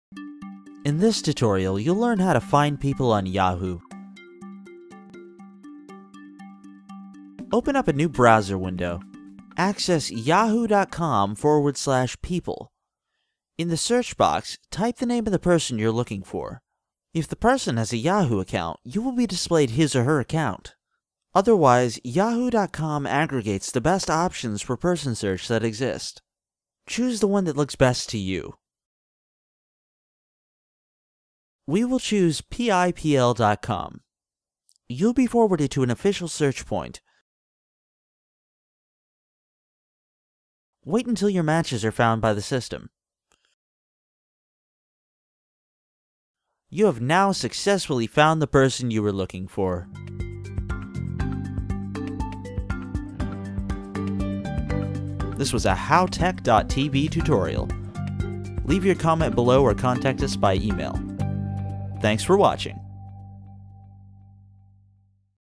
How to Find People on Yahoo
In this tutorial you will learn how to find people on Yahoo.

Step 1. Open up a new browser window. Access

Step 2.   In the search box, type the name of the person you are looking for.

Step 3. If the person has a Yahoo account, you will be displayed his or her account. Otherwise, yahoo.com aggregates the best options for person search that exist.
Choose the one that looks best to you. We will choose pipl.com

Step 4.  You will be forwarded to an official search point; the website announces you that you are about to access real police data.
Confirm, and launch the search. Wait until your matches are found by the system.

Step 5:  Choose the one that is closest to the person you search. You can even get a report on the person.

Result: You have now successfully found the person you were looking for.